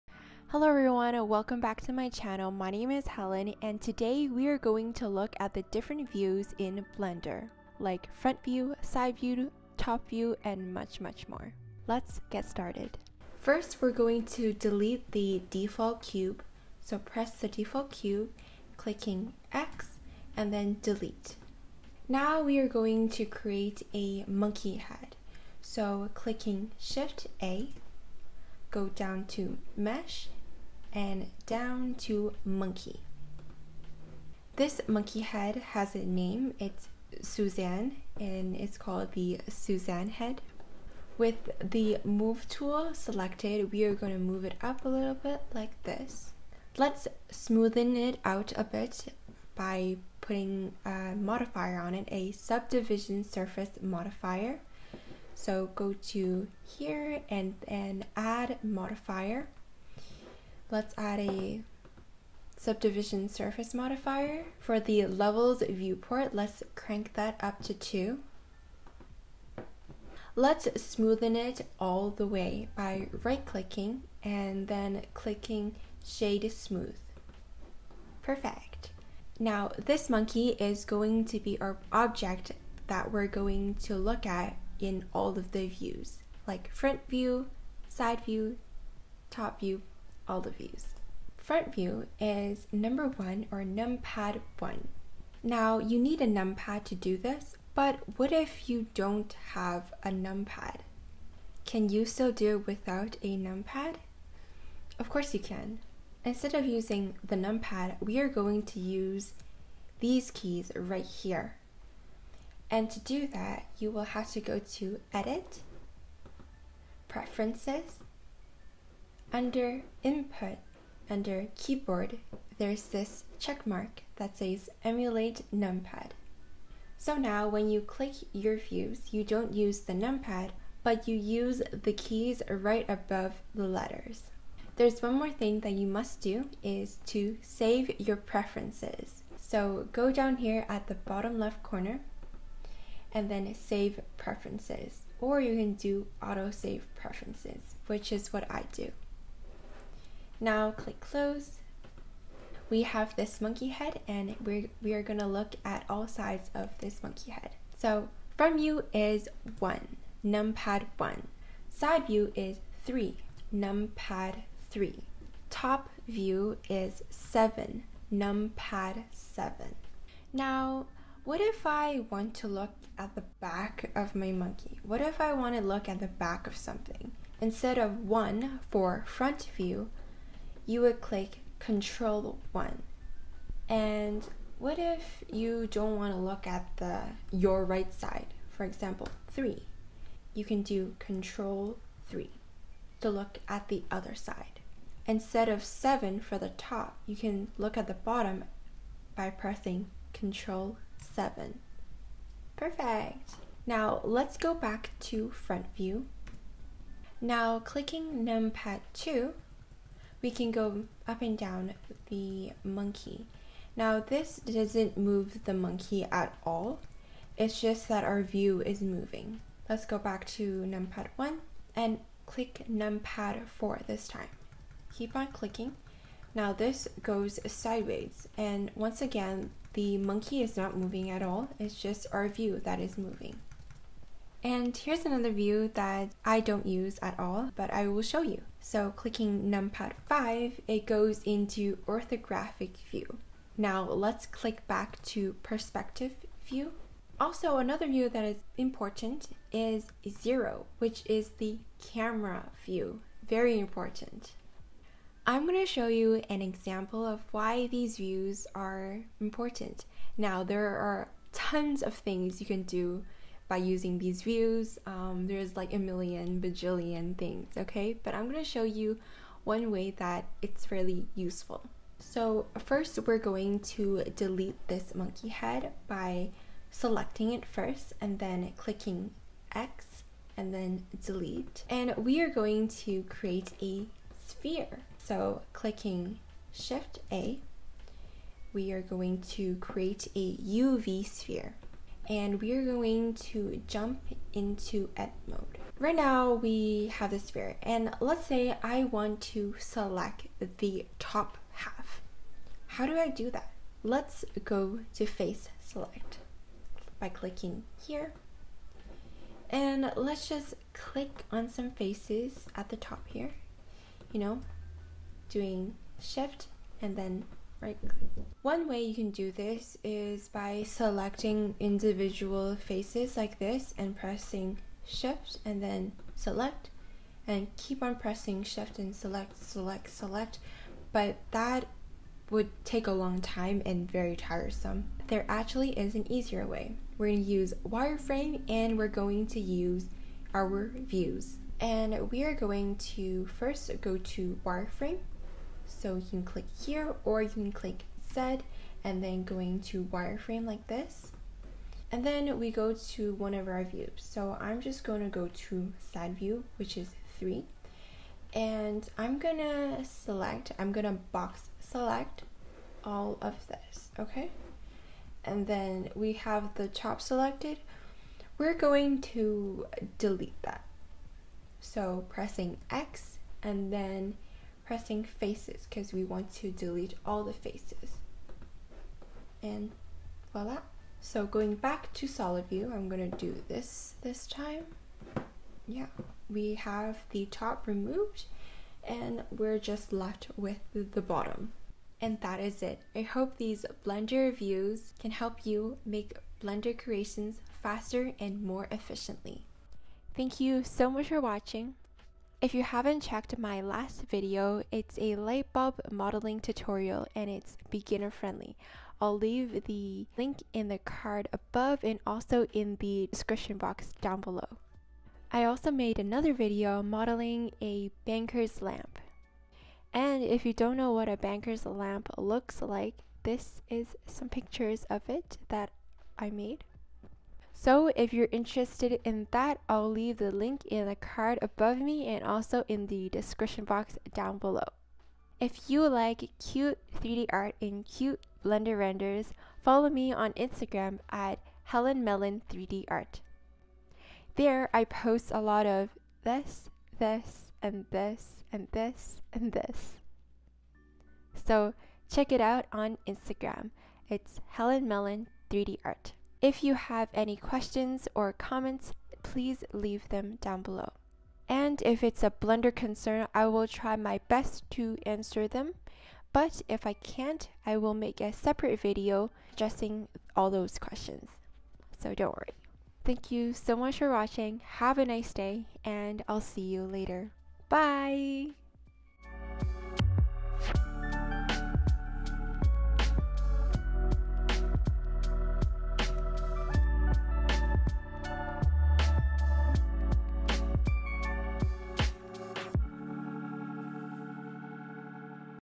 Blender Views | Switching from Front, Side, Top, etc
All about the views in Blender that can help you model faster and with less effort by learning these few shortcut keys.

If you like to see more cute Blender renders or 3d art, you should check my Instagram